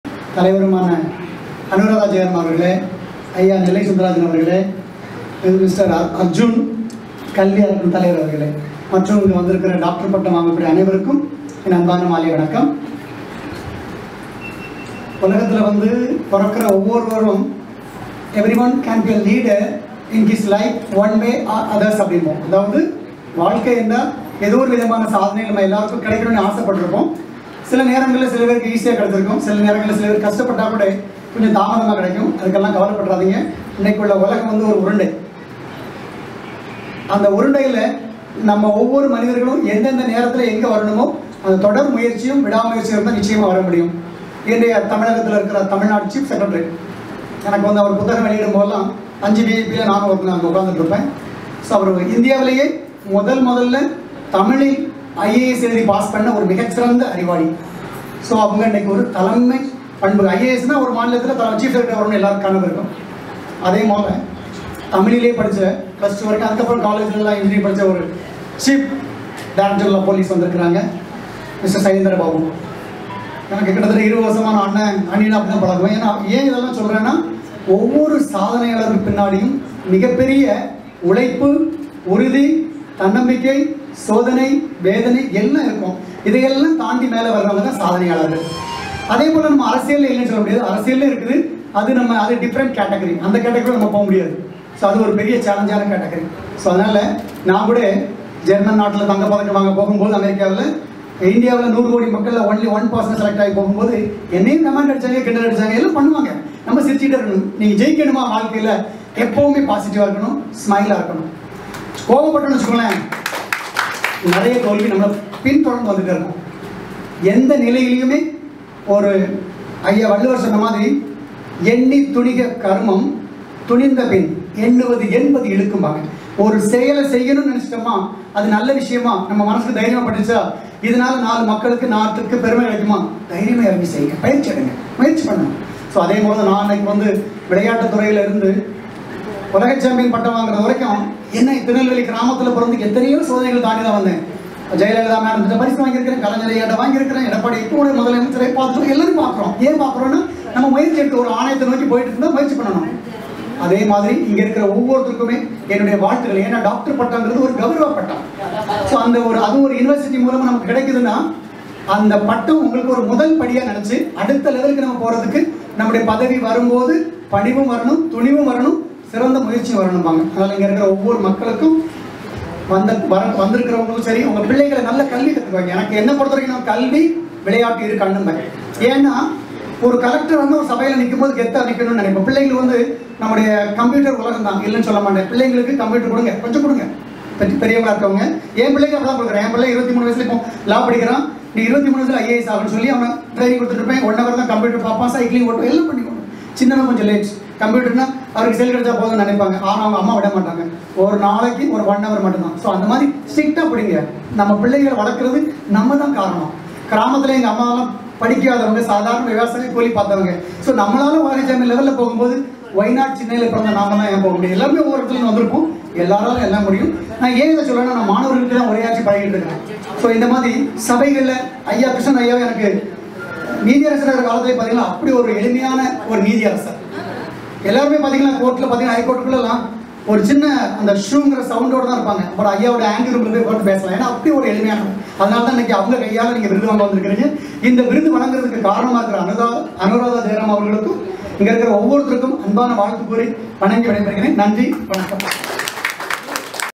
டாக்டர் M.R. சௌந்தரராஜன் டாக்டர் பட்டம் வழங்கிய போது..
# டாக்டர் M.R. சௌந்தரராஜன்

உலகில் பிறக்கும் எல்லோரும் சாதனையாளர்கள் தான்.ஒரு சிலருக்கு இசியாக கிடைக்கும்.சிலருக்கு கஷ்டபட்டு கிடைக்கும்.ஆனால் ஒரு சிலருக்கு மிகவும் தாமதமாக கிடைக்கும்.இந்த உலகத்தில் தொடர் முயற்சியும், விடா முயற்சியும் இருந்தால்
 நீங்களும் வெற்றியாளர் தான்.அதற்கு உங்கள் முன் உள்ள நானே ஒரு உதாரணம்.சாதாரண விவசாய குடும்பத்தில் பிறந்த நான் நூறு கோடி கொண்ட இந்தியா மக்களின் பிரதிநிதியாக விளையாட்டு போட்டியில் பங்கேற்ற போது எத்தனையோ அவமானங்களை, நக்கல் பேச்சுசுகளை ,பல சோதனைகளை கடந்து விடா முயற்சி,நம்பிக்கை துணை கொண்டு உலக அளவில் பரிசு வாங்கினேன்.சிறு ஊரில் இருந்து வந்து என்னால் சாதனை செய்ய முடிந்தது.உங்களாலும் சாதிக்க முடியும் என்பதை நம்புங்கள்.எந்த
செயலாக இருந்தாலும் பாஸிட்டிவ் எண்ணத்தோடும், புன்னகை முகத்தோடும் செயல்படும் போது நிச்சயமாக வெற்றி நம்மை தேடும்.எந்த விதமான எதிர்ப்புகளும் அந்த புன்னகை மூலம் மறைக்கப்பட்டு நாம் வெற்றியாளனாக மாற முடியும் என்பதில் எந்த ஒரு ஐயப்படும் இல்லை.இங்கு டாக்டர் பட்டம் வாங்க வந்த அனைவருக்கும் என் நன்றியை தெரிவித்து கொள்கிறேன்.நன்றி வணக்கம்